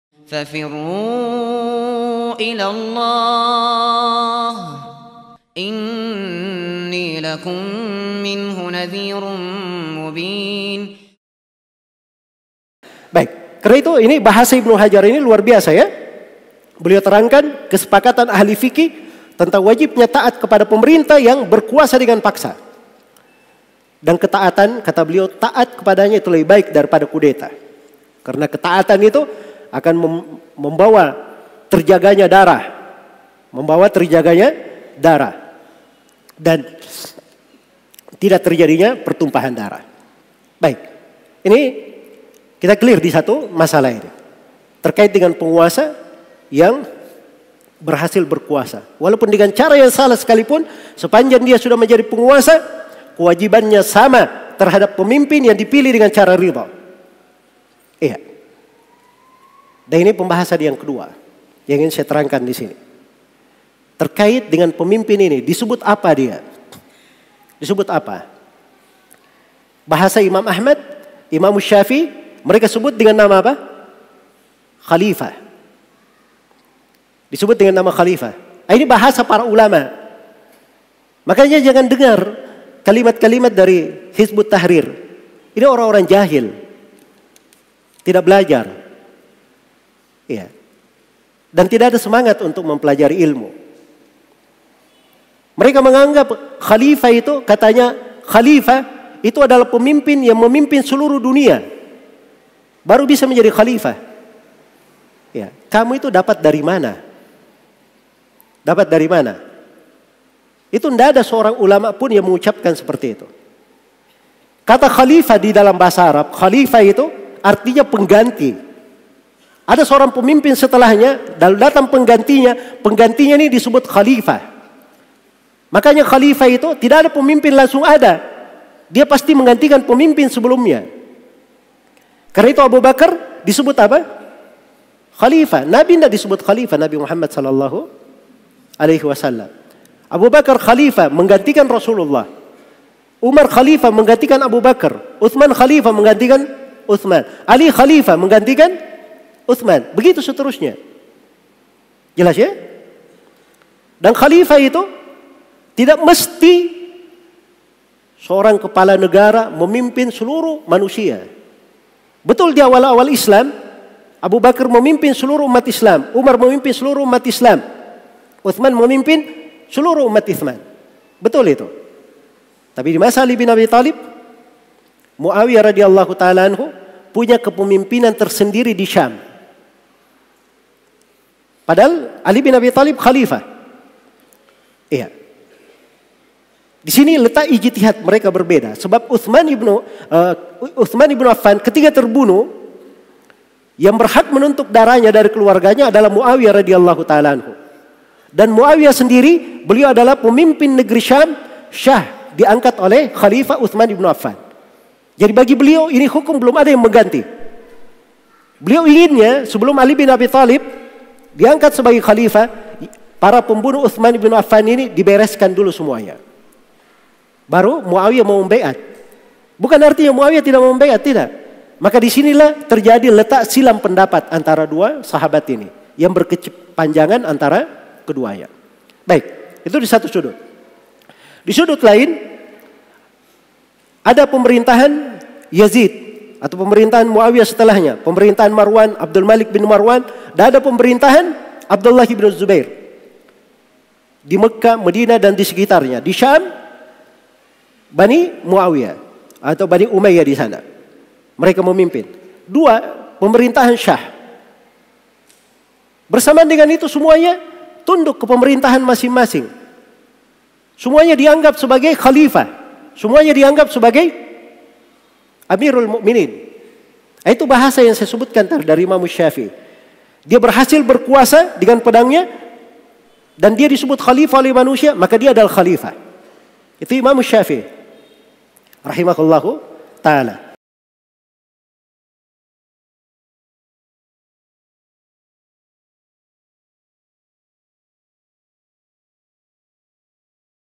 19. Kekeliruan Hizbut Tahrir tentang Khalifah yang Memimpin Umat Islam di Seluruh Dunia
Ini adalah salah satu dari 36 poin pembahasan Tabligh Akbar "Menjawab Kerancuan Pemikiran Penolak Pemimpin Terpilih" yang diselenggarakan pada Ahad, 21 Shafar 1441 atau 20 Oktober 2019 di masjid Muhammad Cheng Hoo Makassar.